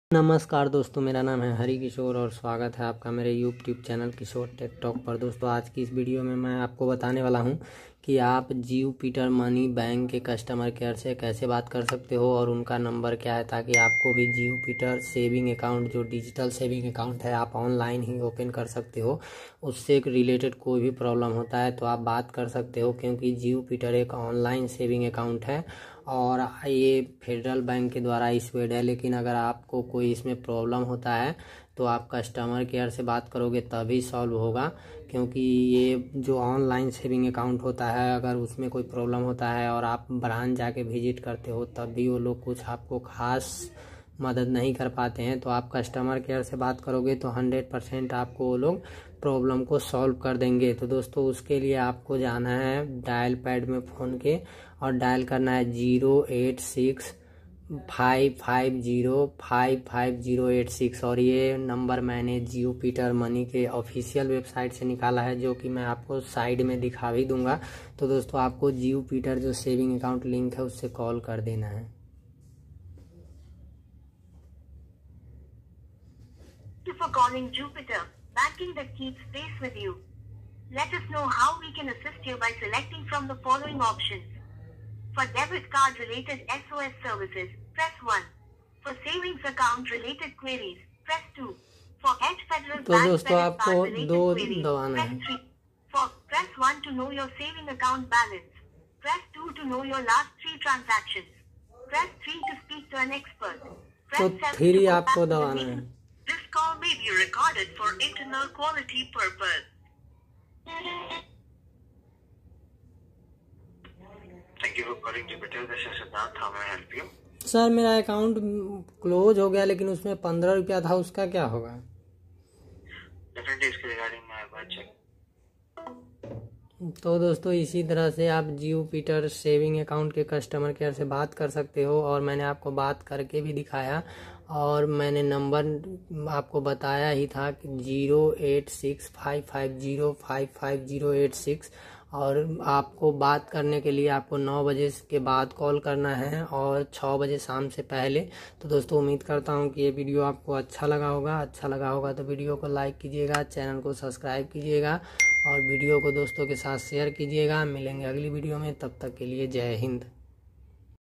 Jupiter saving account customer care number | Jupiter bank account customer care se baat kaise kare
hello friends ish video me maine bataya hai ki aap

Jupiter saving account ke customer care se baat

 kaise kar sakte ho aur maine baat karke bhi

dikhaya hai

video achha lage to video ko like kare

Jupiter Bank customer care toll free number
Jupiter saving account customer care se contact kaise kare

Love you ♥️

Jai Hind ♥️